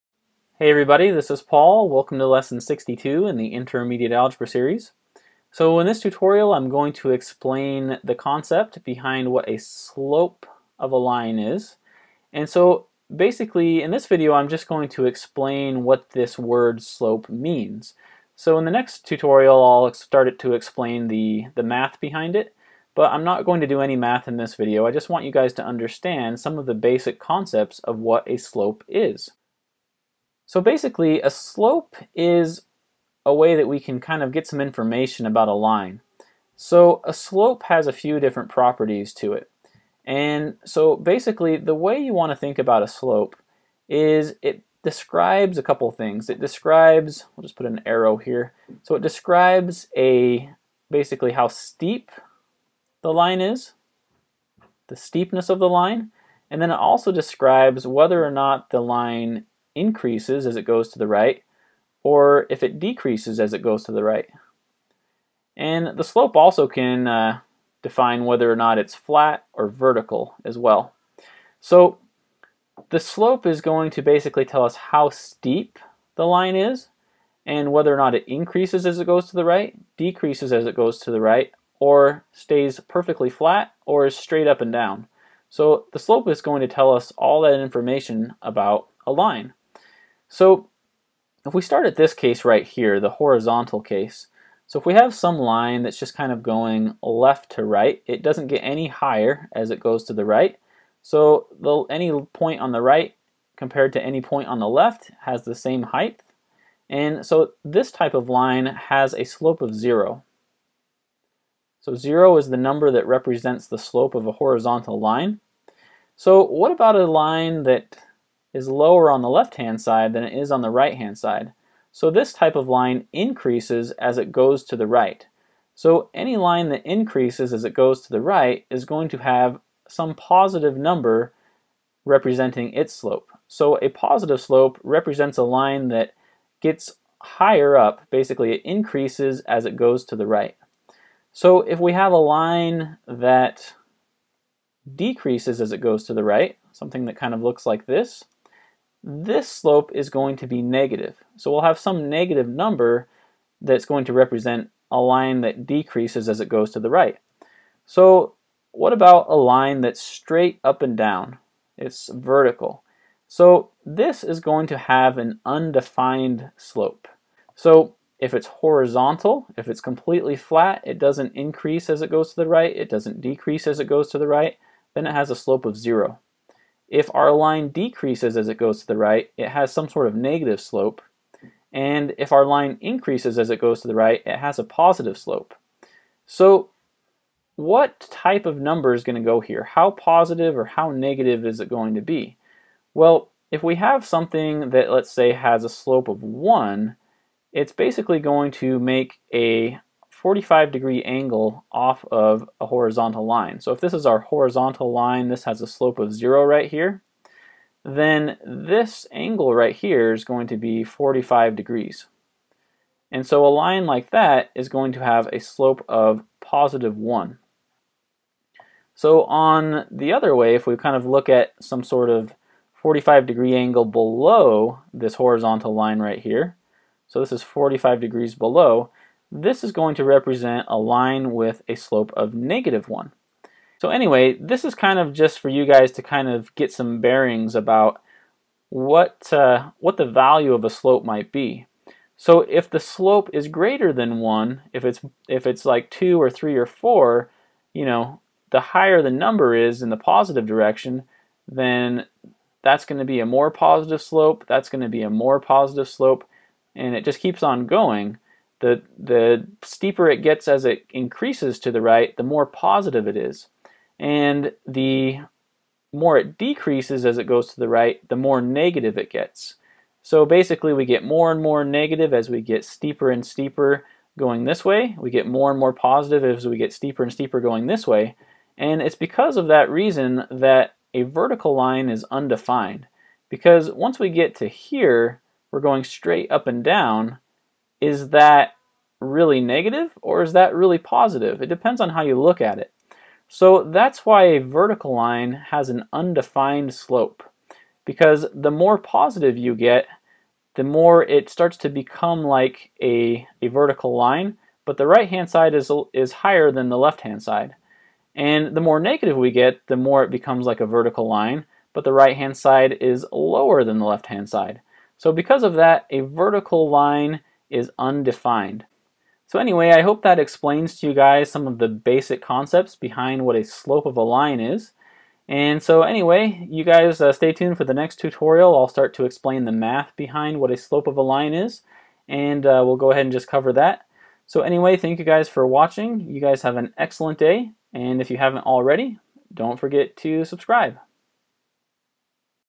What is the Slope of a Line , Intermediate Algebra , Lesson 62
This tutorial explains the basic fundamental concepts about a slope.